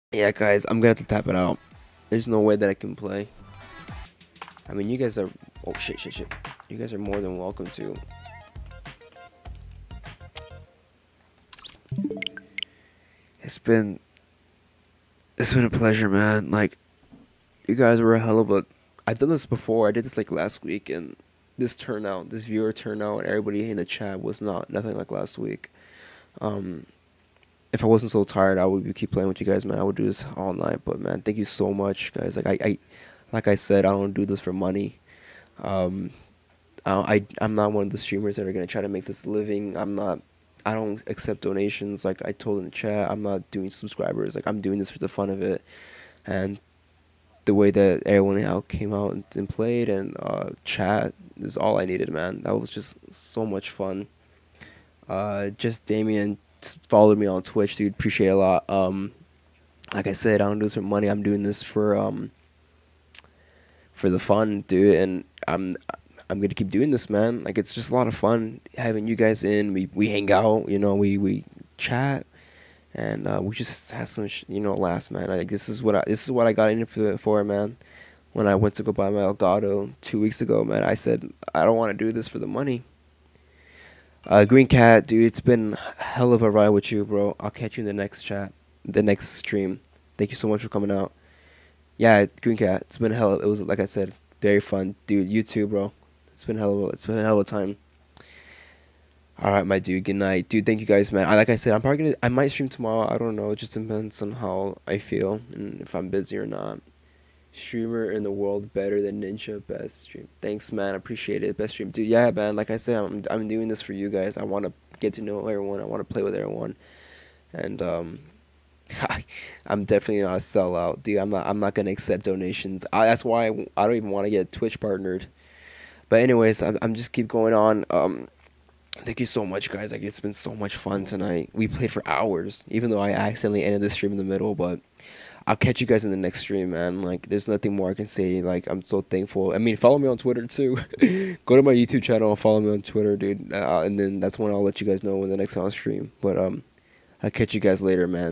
Message To My Followers
My second official stream was last night and the viewer turnout was great. I look forward to doing more and playing more with you all!